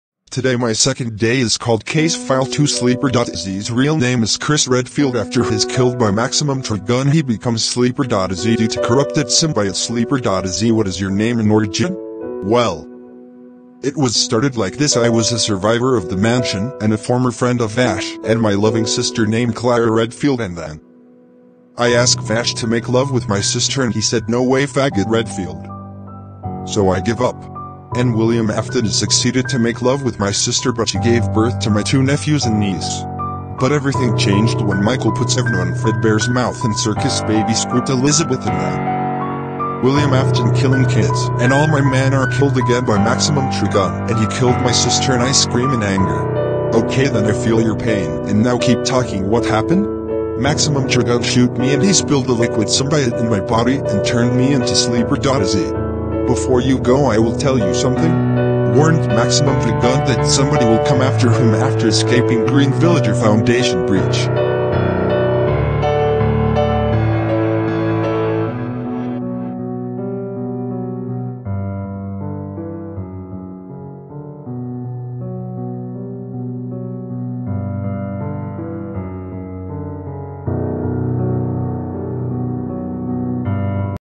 green villager foundation's case files 2: sleeper.exe origin
unlike his wattpad counterpart sleeper.exe real name is Chris redfield he is forcing vash to pregnant his sister but failed but he force William afton to pregnant his sister Clara and than maximum Trigun destroyed the family bloodline for jealous reasons